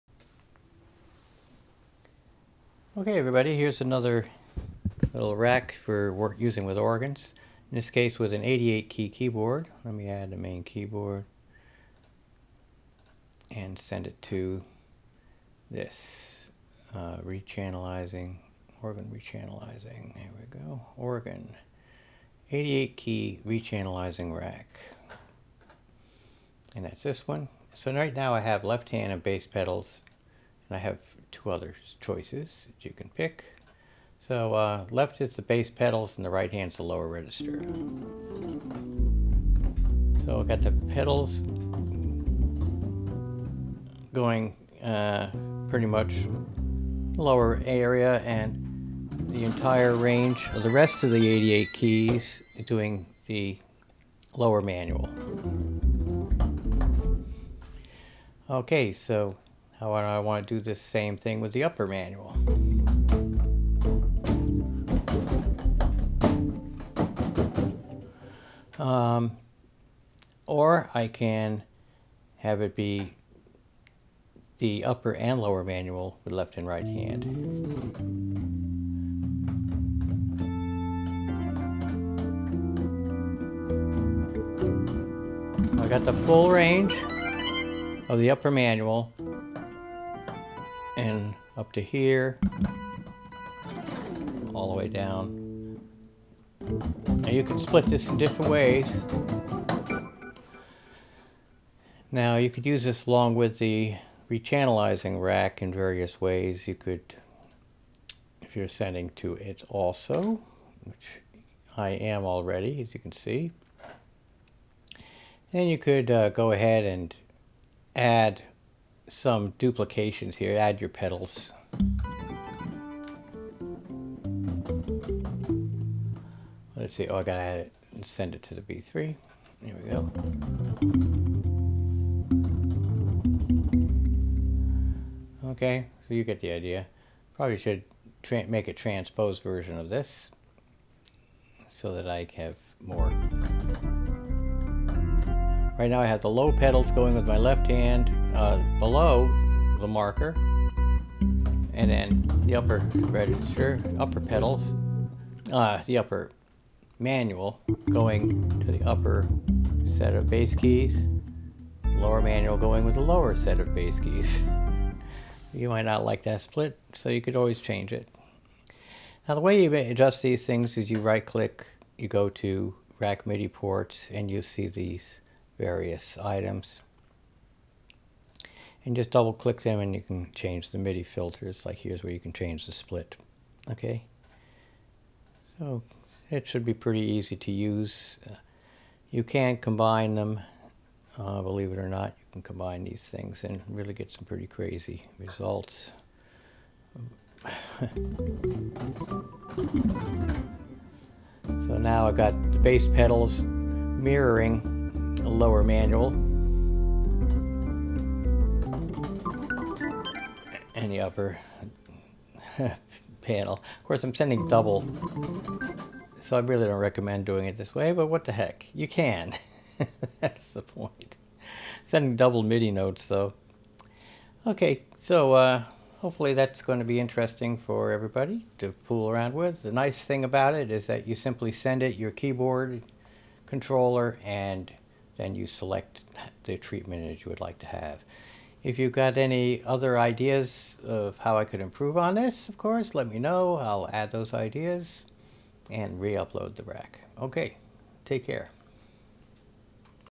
Organ 88 Key Re Channelizing Rack for Cantabile Solo or Performer
This permits using an 88-key keyboard in conjunction with a B-3 style organ emulation to control various parts of the organ.

One mode permits you to control the bass pedal keys with the left hand and the entire lower manual with the right hand.

Another mode permits you to control the bass pedal keys with the left hand and the entire upper manual with the right hand.

The third mode permits you to control the entire upper manual with the right hand and a portion of the lower manual with the left.